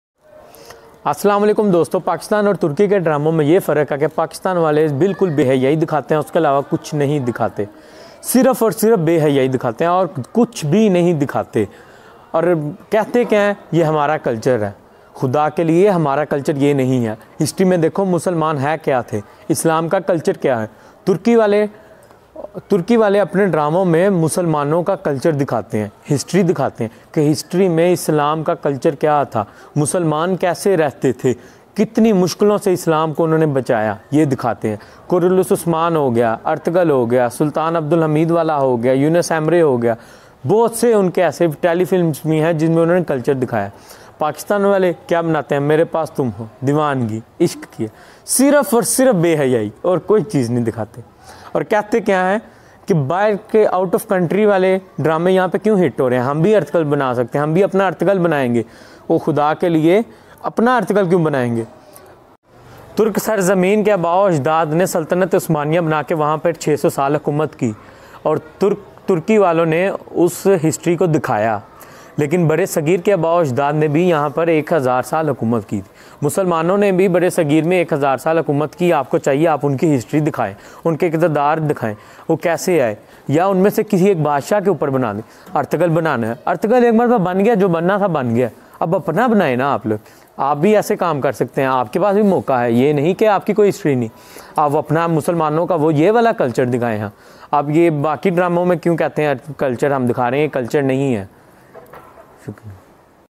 Diffrence between pakistani and turkish dramas||Old video||Syed Ahmad Ali|Ertugrul ghazi
This is the old video of syed Ahmad Ali.
This the Video of previous channel
In this video syed Ahmad is explaining diffrence between pakistan and turkish dramas

and what should Pakistan will make instead of Ertugrul Ghazi...